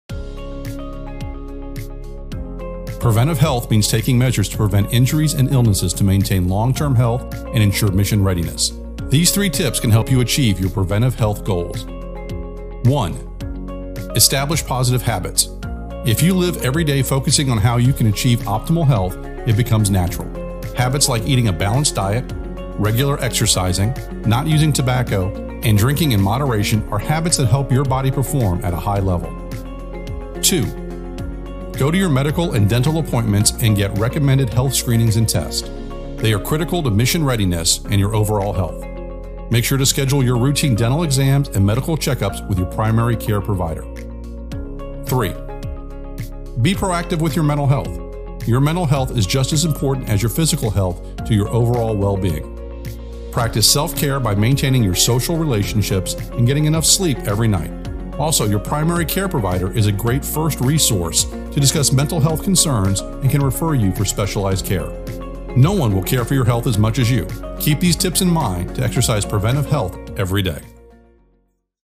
Preventive Health Care for You
Preventive health care - getting a physical exam, getting vaccines on time, and listening to your body - is key to a long and healthy life.